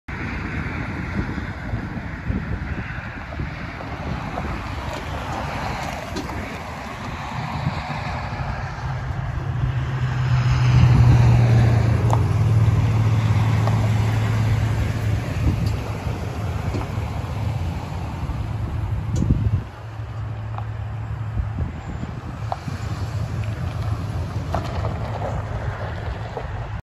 2014 Dodge Ram 2500 HD Crew Cab pickup truck for sale at auction | bidding closes February 9, 2022
2014 Dodge Ram 2500 HD  Crew Cab pickup truck
 VIN: 3C6TR4CT6EG258176
Engine
 Displacement: 5.7L V8 OHV 16V
 Cylinders: 8
 Fuel type: Gas
Transmission
 Automatic
Interior
 AC, Heat
 Heated mirrors
 Power windows, Power locks
 Cruise control
Tires
 Size: LT295/60R20
Notes
 Passenger side mirror cracked
Oklahoma title